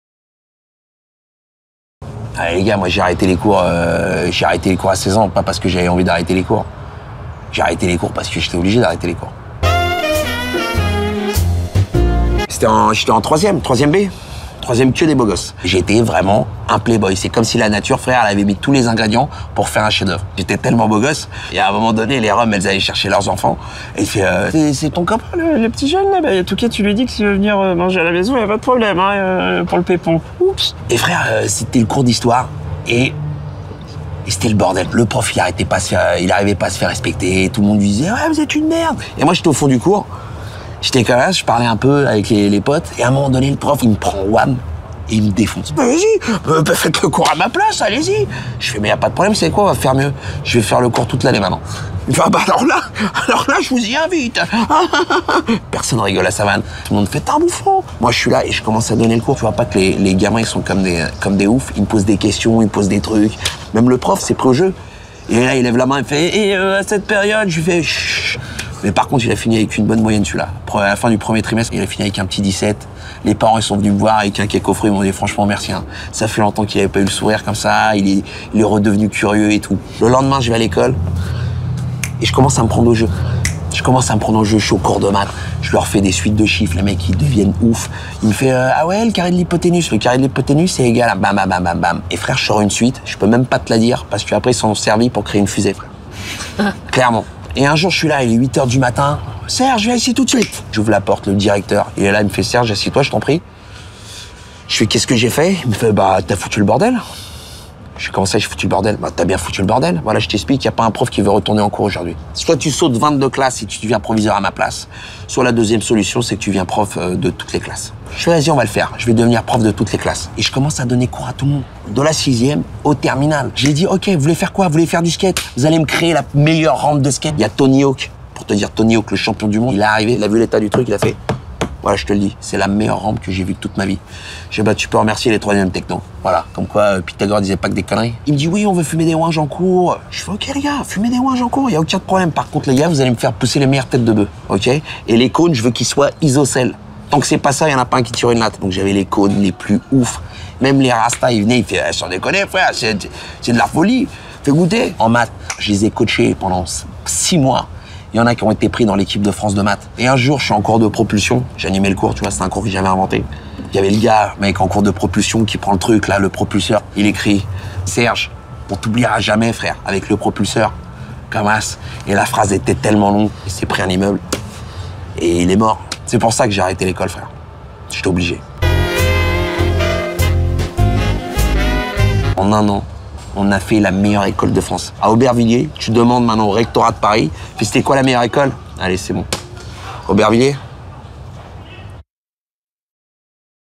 Serge Le Mytho #01 - Pourquoi Serge a arrêté l'école ?
L'histoire d'un mec qui raconte des histoires.